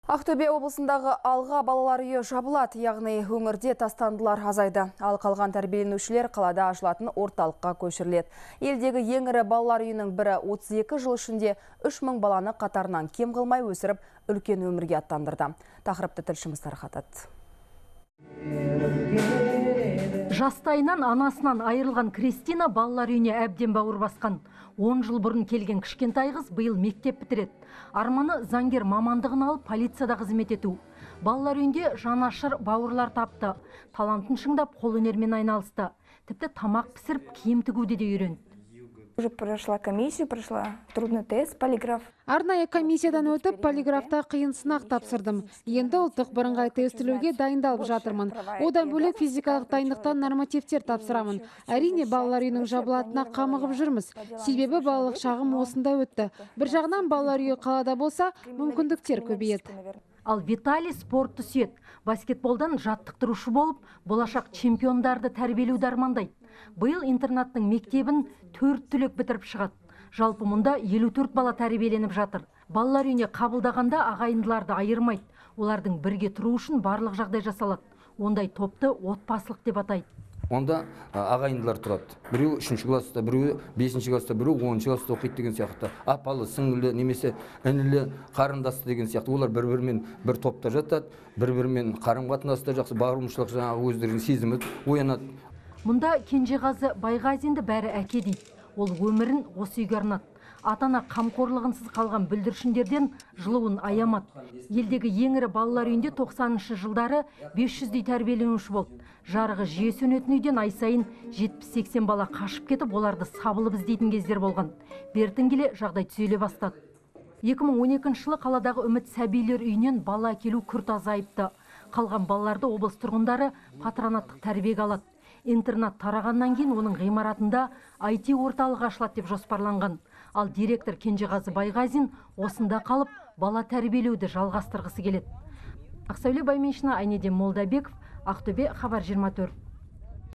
Ақтөбе облысындағы «Алға» балалар үйі жабылады
Ақтөбе өңірінде тастандылар азайды.

Ал қалған тәрбиеленушілер қалада ашылатын орталыққа көшіріледі. Елдегі ең ірі балалар үйінің бірі 32 жыл ішінде 3 мың баланы қатарынан кем қылмай өсіріп, үлкен өмірге аттандырды.

Жастайынан анасынан айырылған Кристина балалар үйіне әбден бауыр басқан. 10 жыл бұрын келген кішкентай қыз биыл мектеп бітіреді. Арманы – заңгер мамандығын алып, полицияда қызмет ету. Балалар үйінде жанашыр бауырлар тапты. Талантын шыңдап, қолөнермен айналысты. Тіпті тамақ пісіріп, киім тігуді де үйренді.